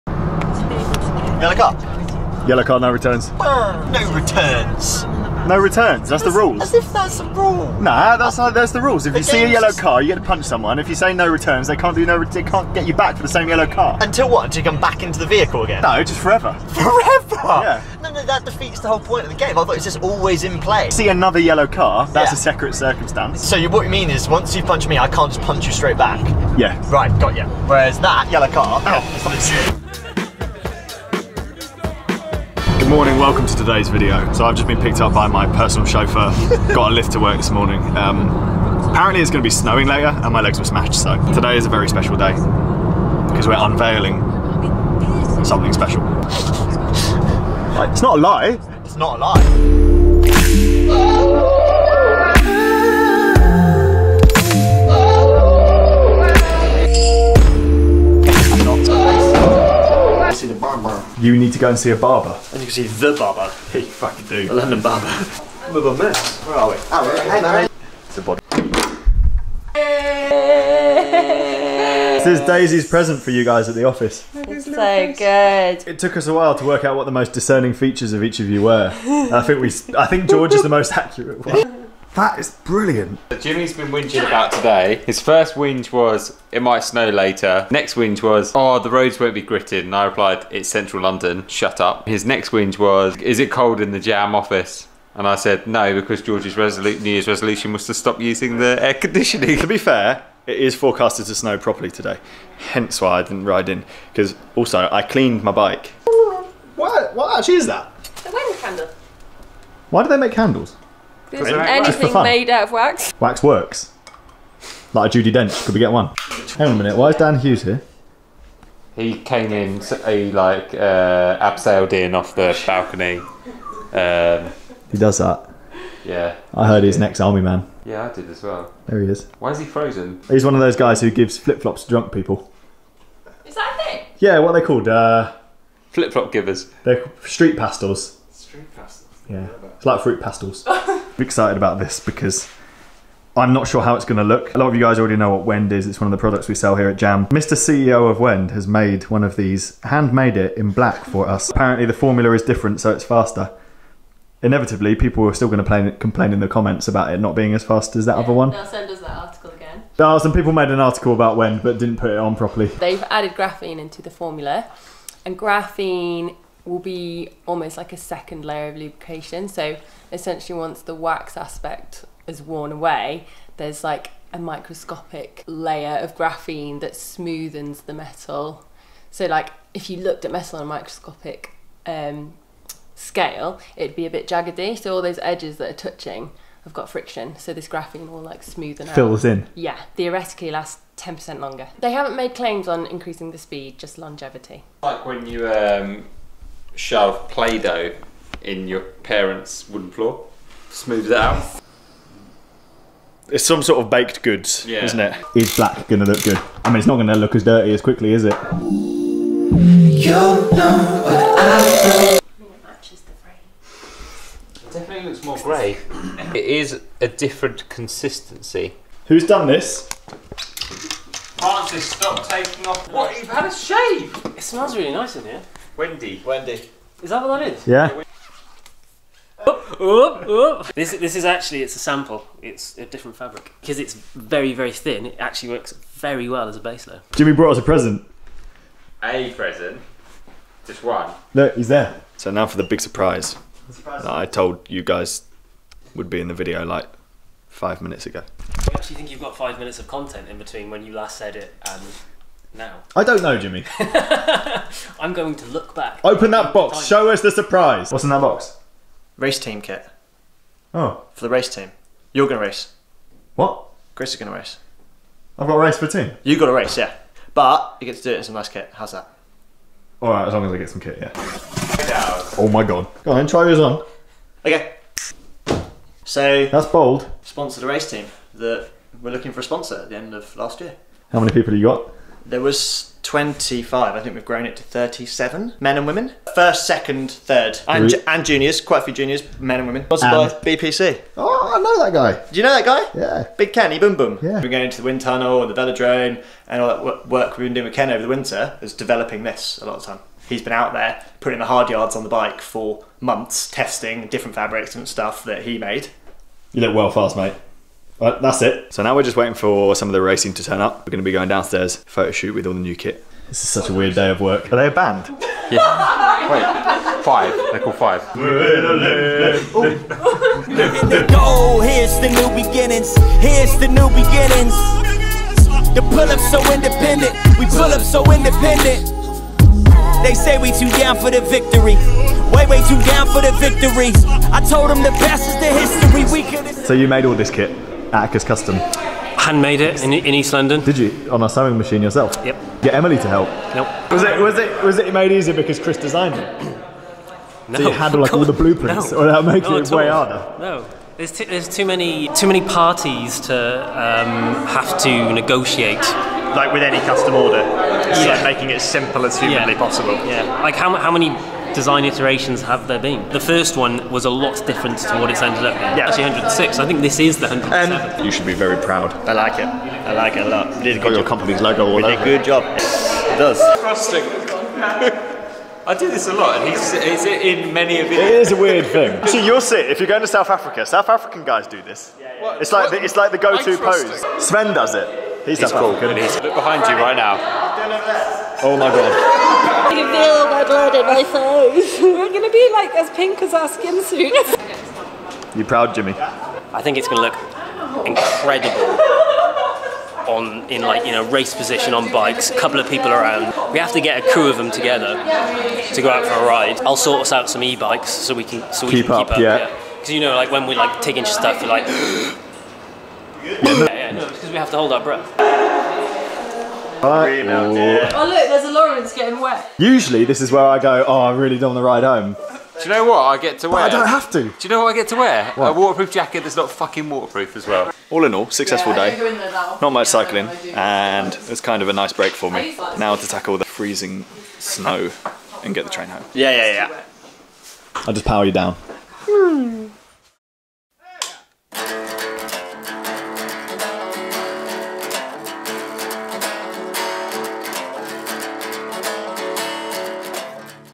MY NEW RACE TEAM
my work, JAM cycling have sponsored the BPC racing team for 2019, they gave me a skinsuit so now I have to race.

Jam Cycling,
Attacus Cycling,
Wilier Triestina,
MET Helmets,
Shimano,
SpinOnThese Wheels,